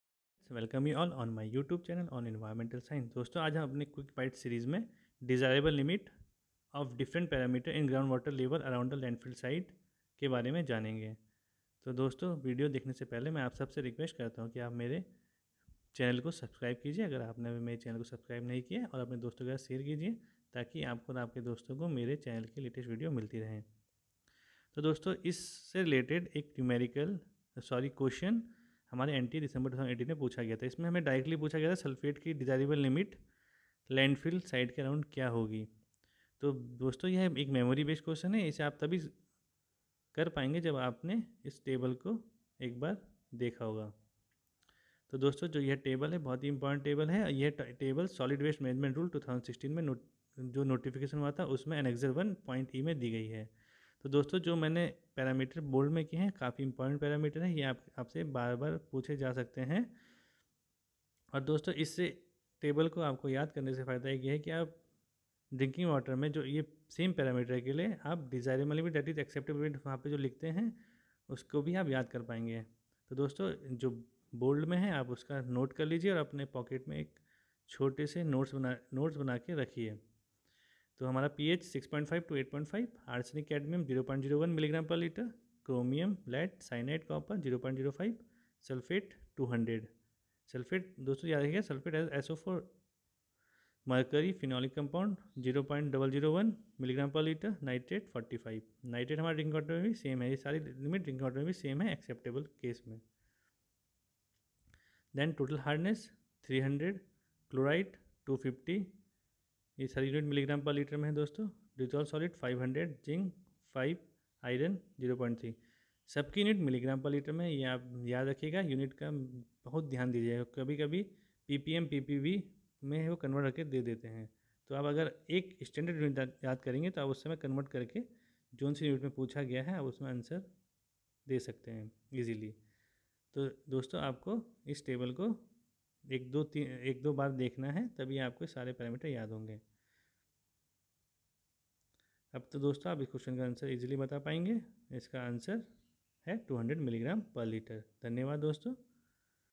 Desirable Limit of various parameter in Ground Water Level around the Landfill Site
Question: According to IS 10500:2012, the desirable limit of sulphates as SO4 in ground water around the landfill site for wastes is  [NTA-Dec’18]

1. 200 mg/l
2. 300 mg/l
3. 400 mg/l
4. 250 mg/l